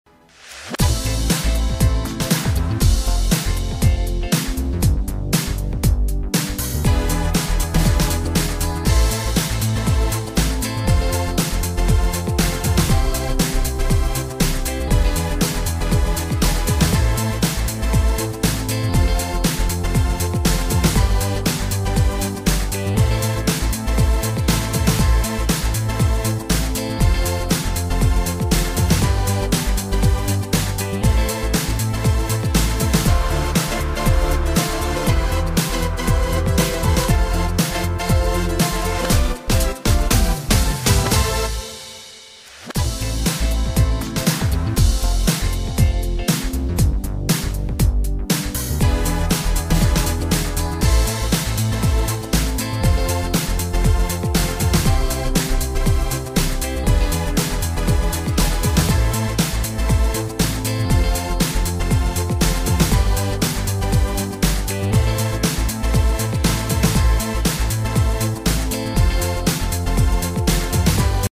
Shangtui Se205 Hydraulic Excavator performance display#shantui
SE220 hydraulic excavator, equipped with strong and reliable engine power system, lower fuel consumption, energy saving and environmental protection; Equipped with imported hydraulic parts, the quality is stable and reliable, and the work efficiency is high; The advanced domestic intelligent electrical system and high-strength structural parts are adopted to enhance the adaptability to working conditions and make it more energy-saving, efficient and environmentally friendly. The embedded GPS system in the intelligent information terminal improves the intelligent level of the whole machine and effectively ensures the safety of customer assets.